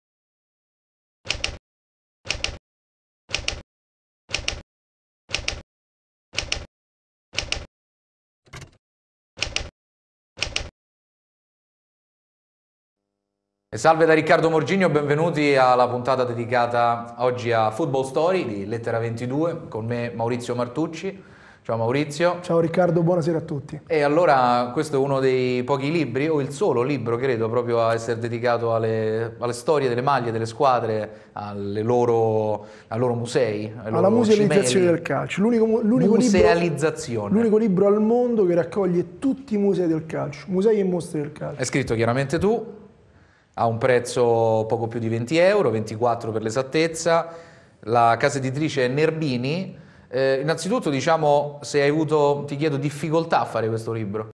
lettera 22.mpg
Riccardo Morgigno ci porta a conoscere Football Story il libro di Maurizio Martucci dedicato alle storie delle maglie delle squadre e alla musealizzazione del mondo del calcio